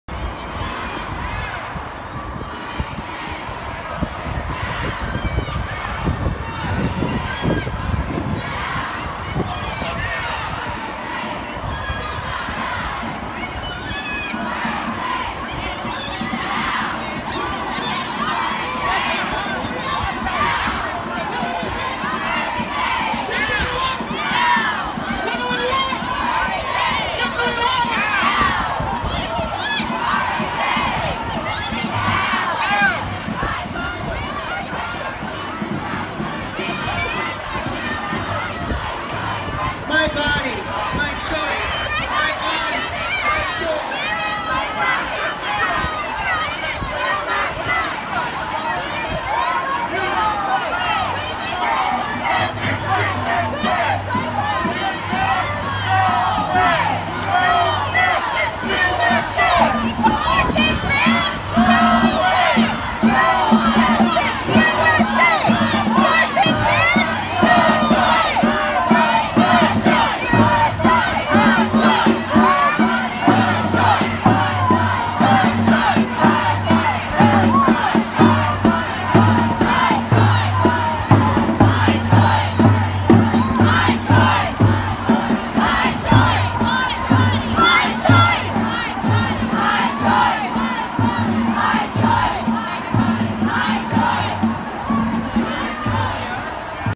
Emergency Rally to Save Reproductive Rights (March along South Dearborn Street)
Protesters marched from Federal Plaza to Thompson Center